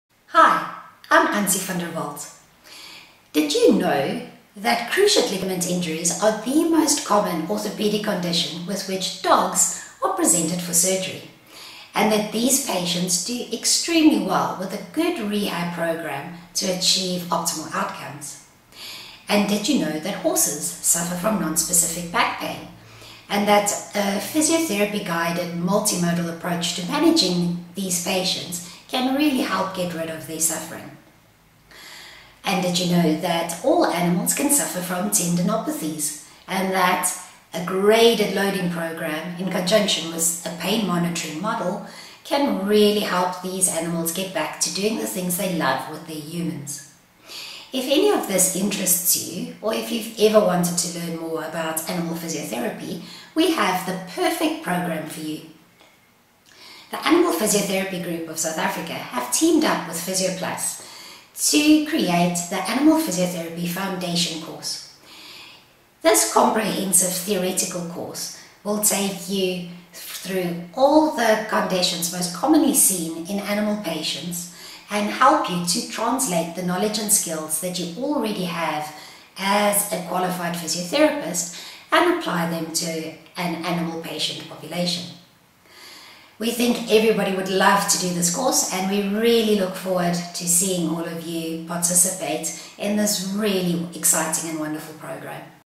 Animal Physiotherapy Course | 30 hours+ of Videos Available on Physioplus
The most comprehensive programme of courses for animal physiotherapists anywhere online.

Animal physiotherapy is a well-established branch of physiotherapy and we are proud to have teamed up with the Animal Physiotherapy Group of South Africa to create this incredible 30 hour programme.

The physiotherapist can play an important role in promoting animal health and wellness. However, the ability to be both safe and effective as an animal physiotherapist depends on a deep understanding of the differences between human and non-human patients in terms of anatomy, biomechanics, assessment and treatment approaches.

The treatment of animals brings with it unique challenges, and the safety of the physiotherapist, the patient and the animal’s handler is paramount. This foundation course will equip the qualified physiotherapist with the theoretical basis to translate their knowledge and skills to an animal patient population.